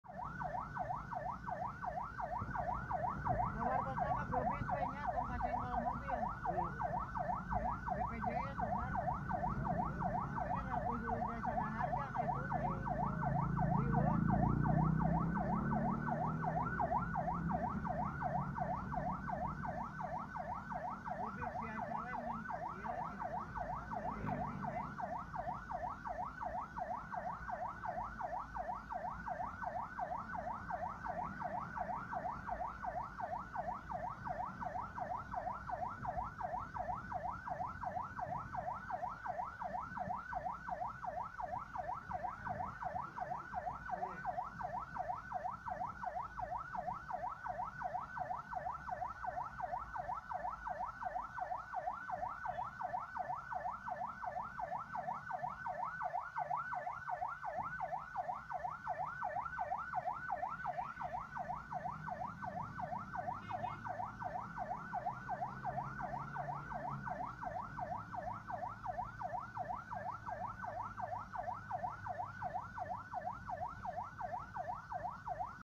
Respect to sopir Ambulance
Respect kepada para pengendara Mobil dan motor menghormati dan and menghargai bapak sopir ini saat membawa Mobil ambulance Mantaap